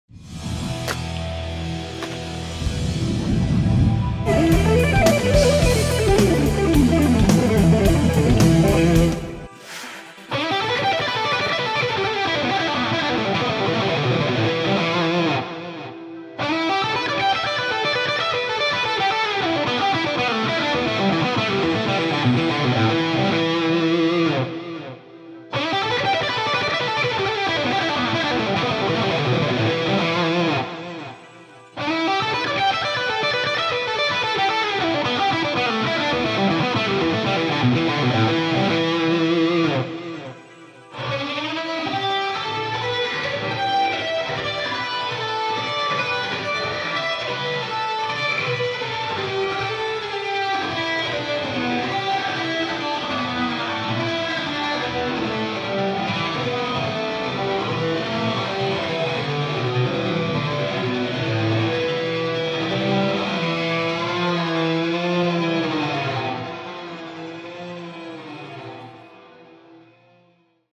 Eric Johnson: Pentatonic I-III Cascade
The Eric Johnson pentatonic I-III cascade and its origin in a classic performance.